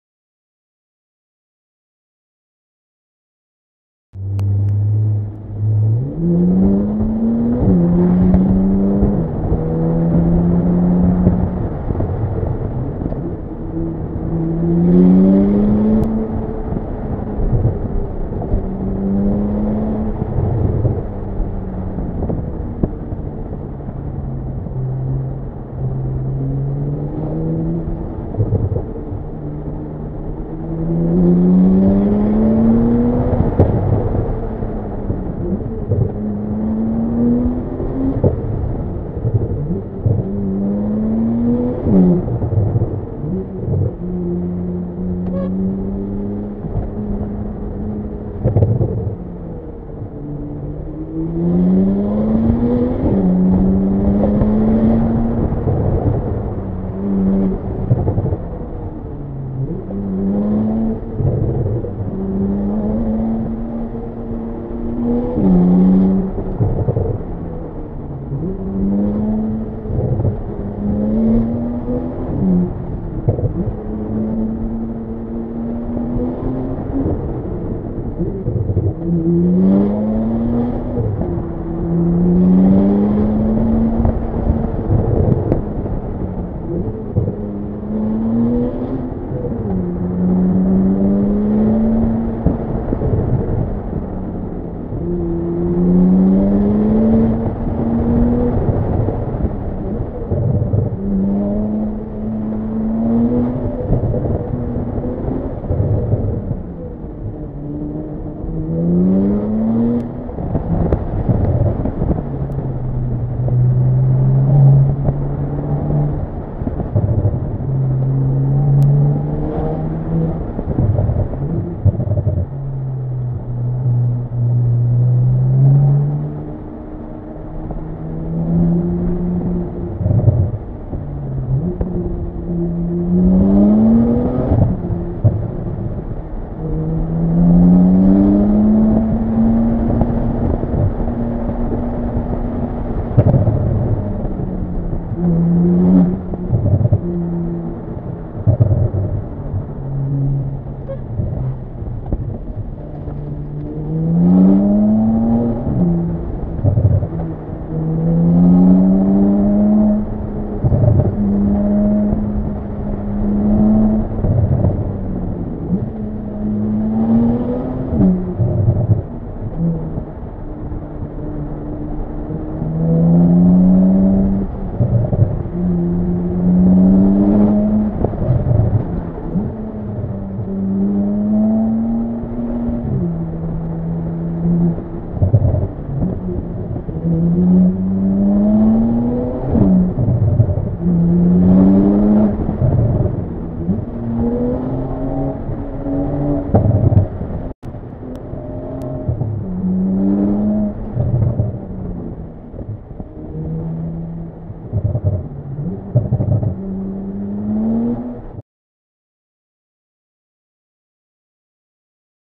991 Turbo S in the Canyons
Getting use to the 991 Turbo S in the canyons with my new exhaust...I'm not too crazy about the off throttle fuel burble popping. ehh
This video has been edited for the illusion of speed...